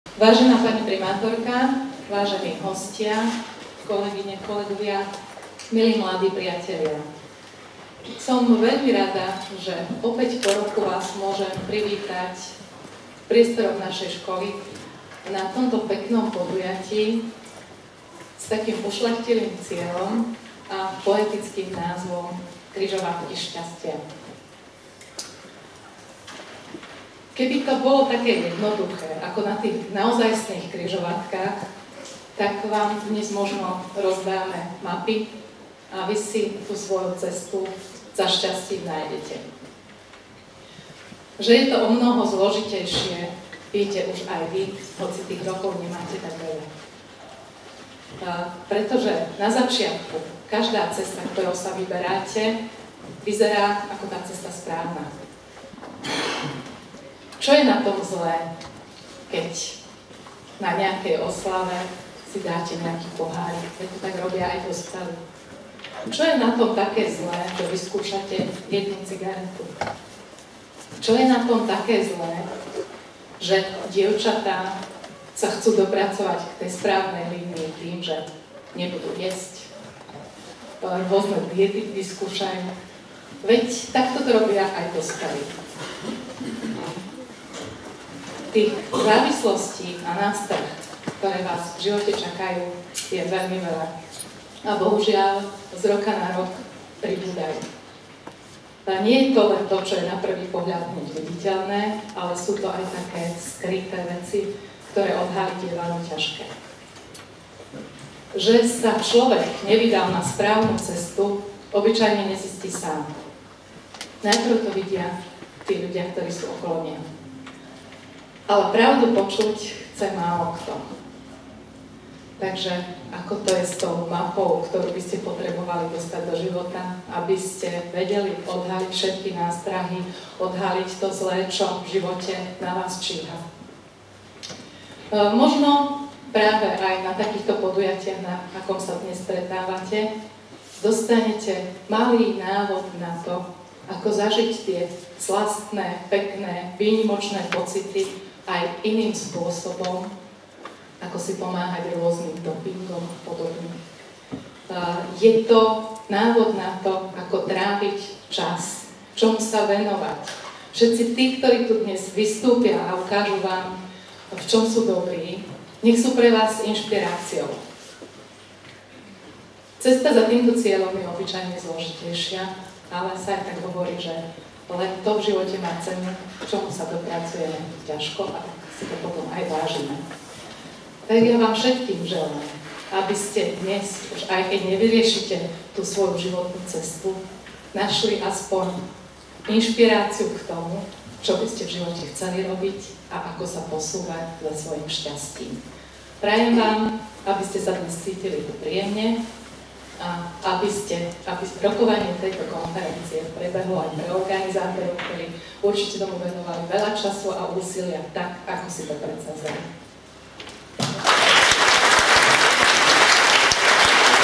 Mgr. Ľubica Tomčíková - Križovatky šťastia 2015
V aule ZŠ na sídlisku Západ sa konala detská konferencia pod názvom Križovatky šťastia 2015